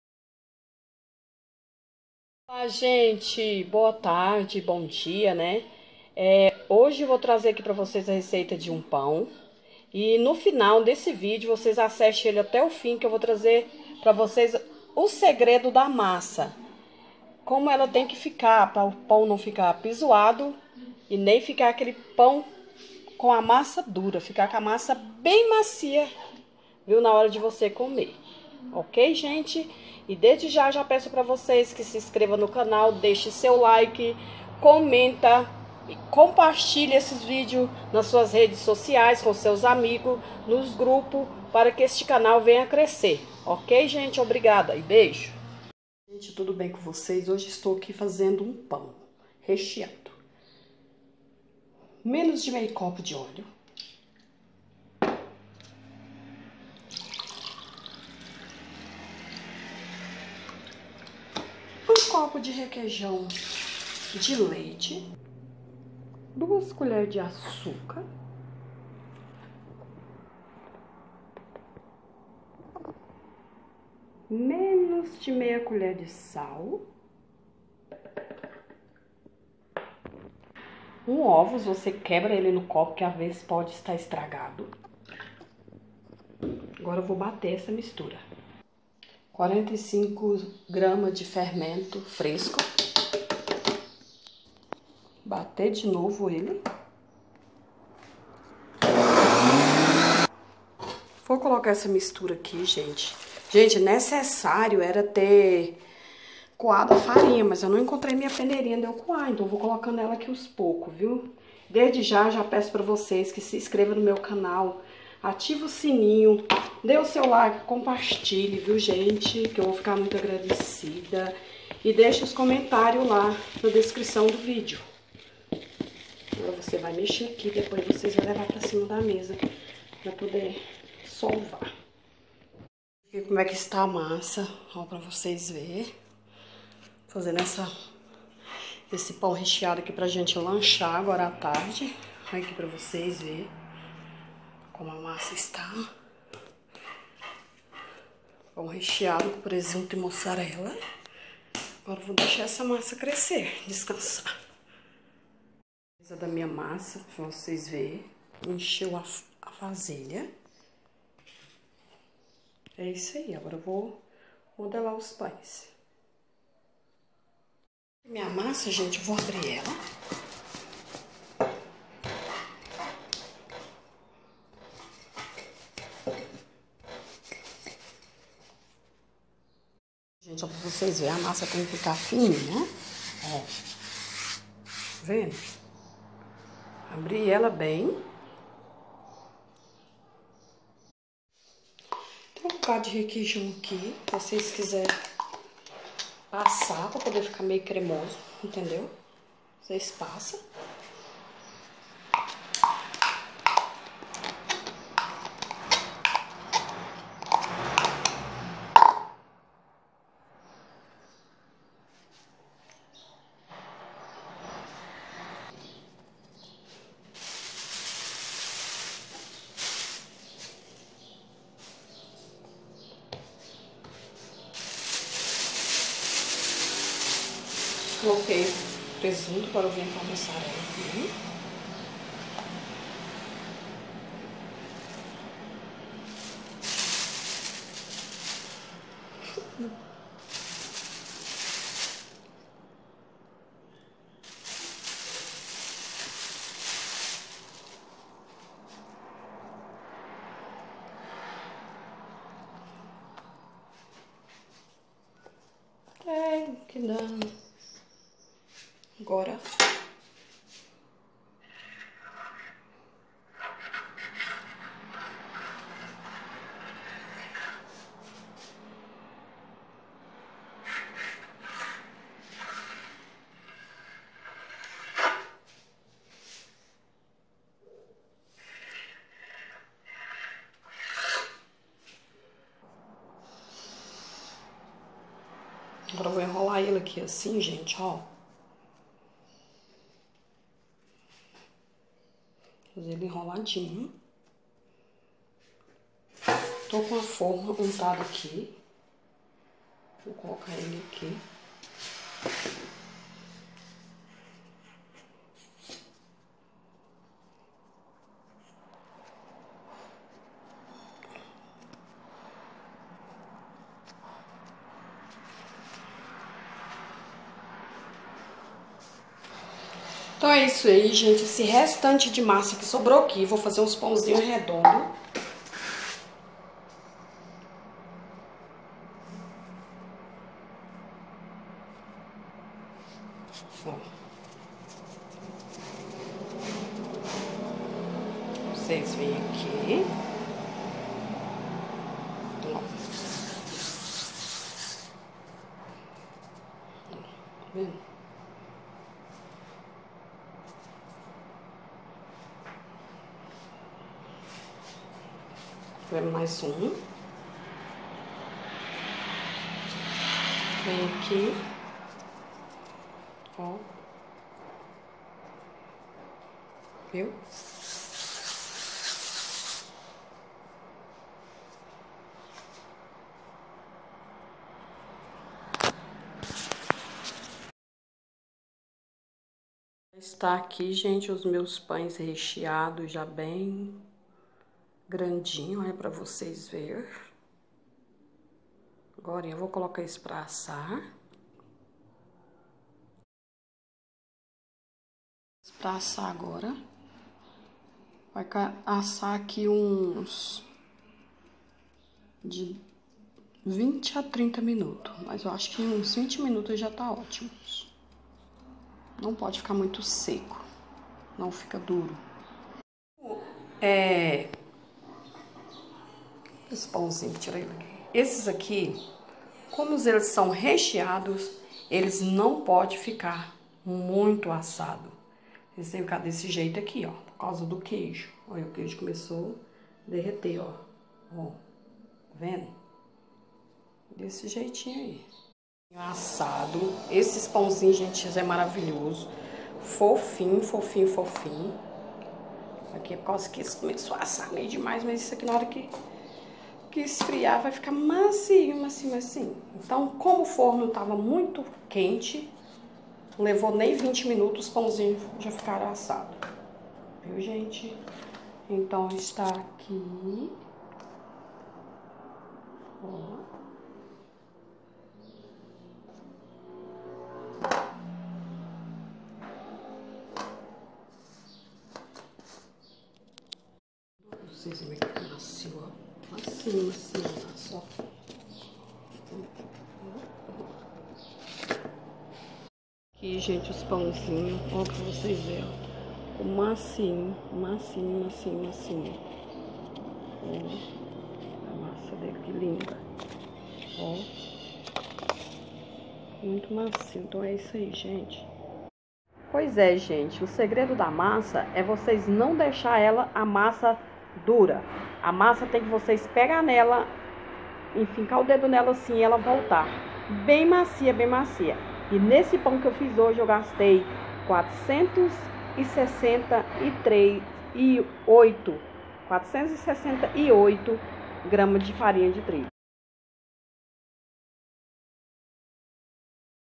Pão caseiro recheada
1 copo de leite
2 colher de açúcar
1/2 colher de sal
1/2 copo de óleo
45g de fermento
470g de farinha de trigo.

Modo de preparar em uma bacia você coloca o líquido e vai colocando a farinha de trigo aos poucos mexa bem depois leva para cima da mesa e a massa por 10 minutos depois leva para descansar uma hora modele os pães e deixa descansar mais uma hora